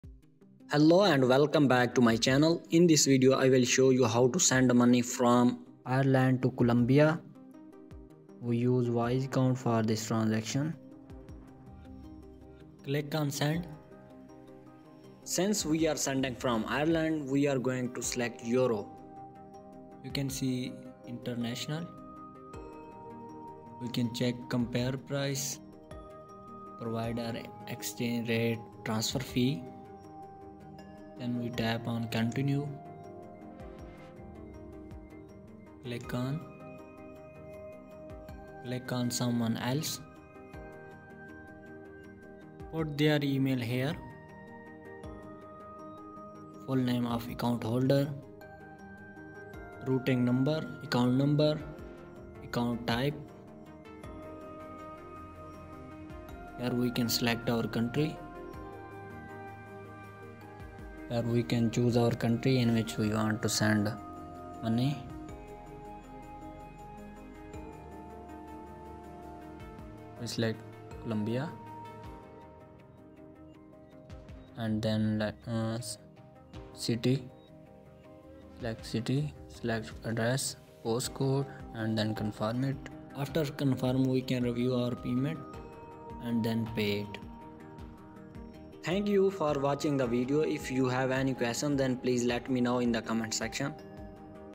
✅ How To Transfer Money From Ireland to Colombia on WISE.com (Full Guide)
Embark on a seamless international money transfer journey from Ireland to Colombia with our comprehensive guide on using WISE.com. We provide a step-by-step walkthrough, ensuring you grasp the process effortlessly. Whether it's for personal matters or business transactions, WISE.com offers a secure and cost-effective solution.

Key Highlights:
🌐 Step-by-step tutorial for money transfer on WISE.com
💸 Cost-effective solutions for international transactions
📈 Understanding currency exchange rates
🔒 Ensuring the security of your transactions with WISE.com

Make your cross-border transactions stress-free and economical. Dive into our guide now and simplify the process of sending money between Ireland and Colombia.